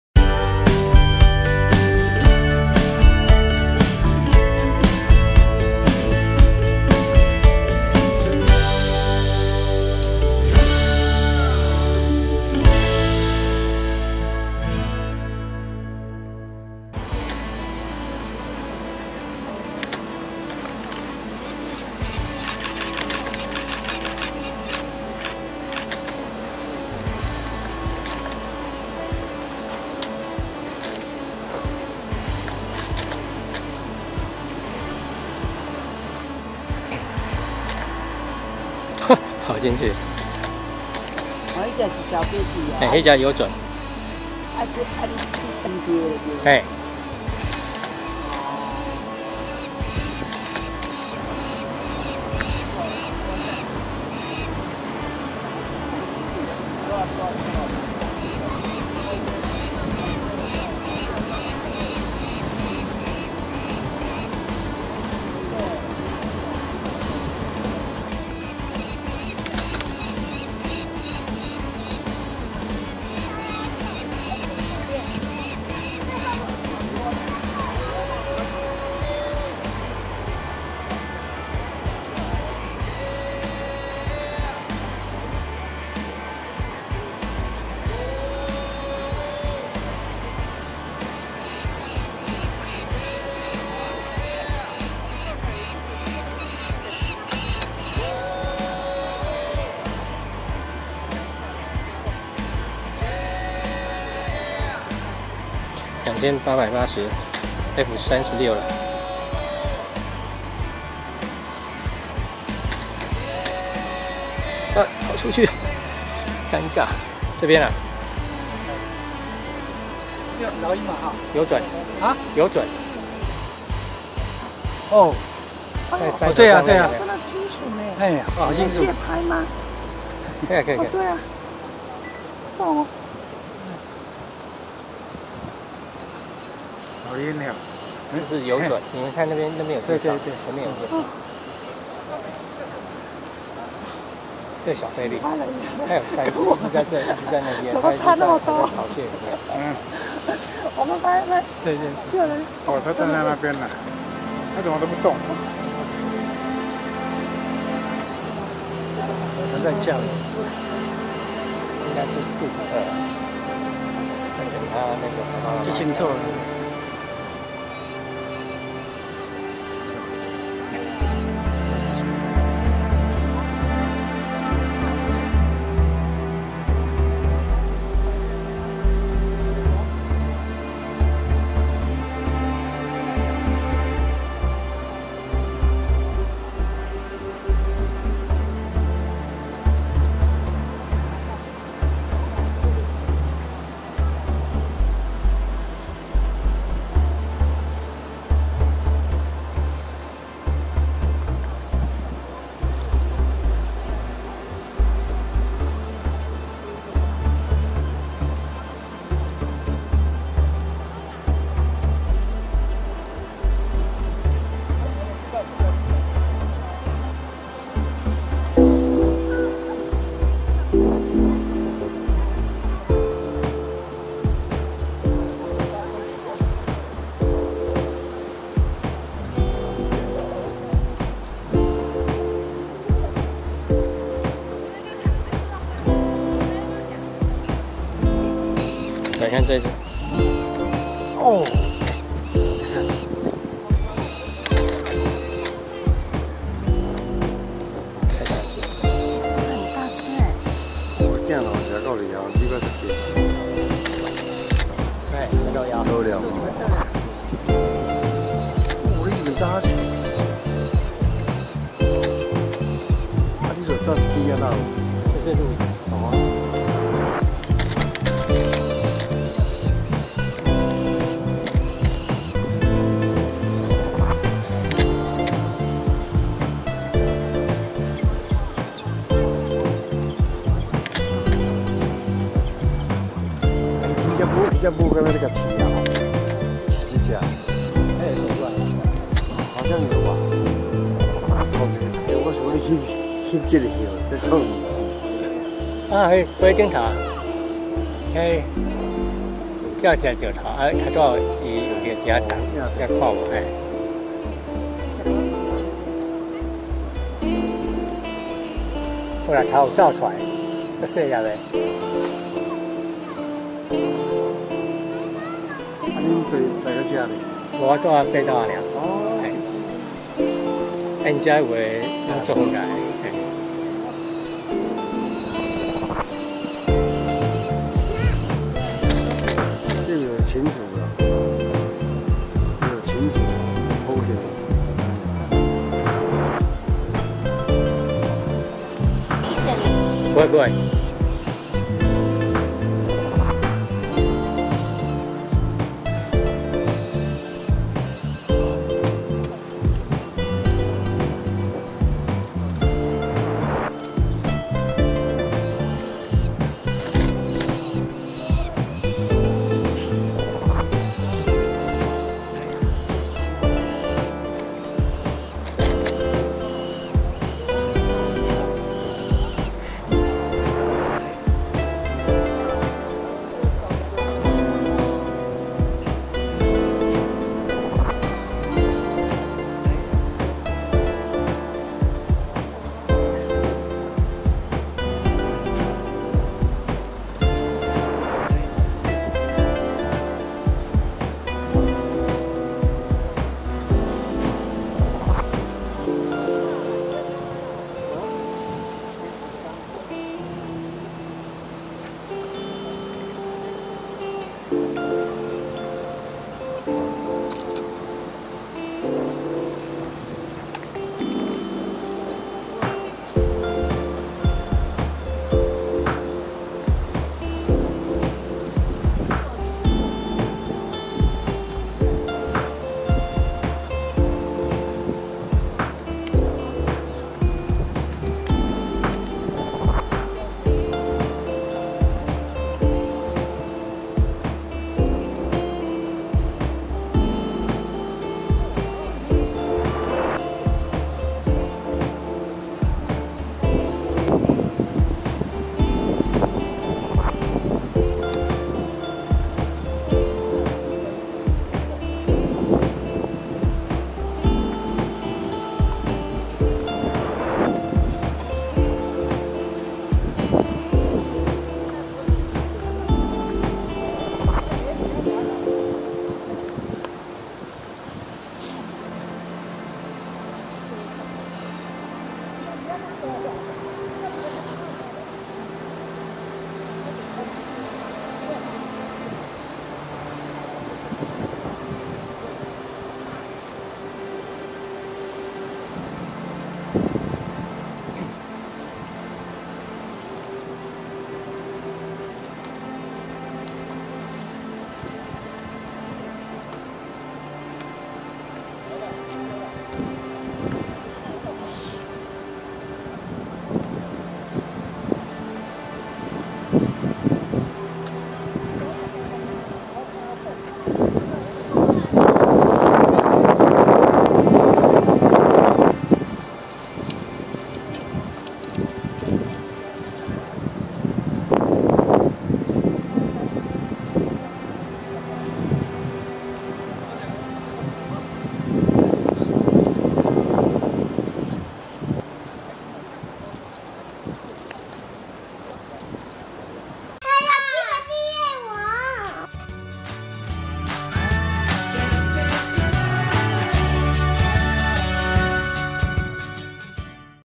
酋長岩遊隼_EP25_二寶站在高崗上_200509
2020年5月9日遊隼二寶已經飛回到山壁上,
今天光線很漂亮,
遊隼一家也表演的很精采.
這一集收錄了二寶站在山頭上的片段,
陽光剛好灑下來,整個畫面非常漂亮.